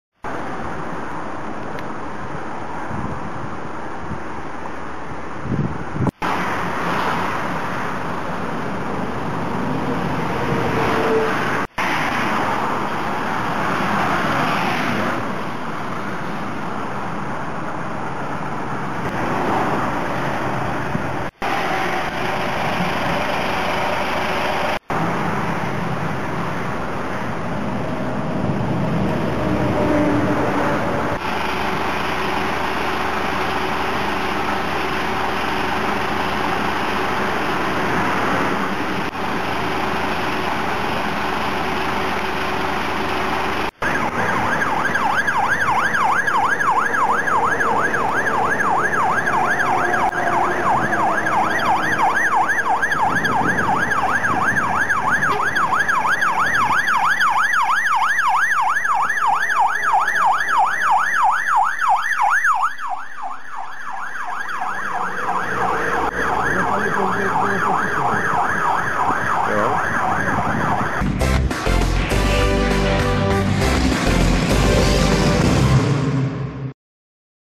Dálniční ulička - pro motorkáře ....
Dálniční ulička pro záchranáře v české praxi - vhodná pro policisty a záchranáře motorkáře .:-))
A co ty haldy kamiónů v  levém jízdním pruhu ...??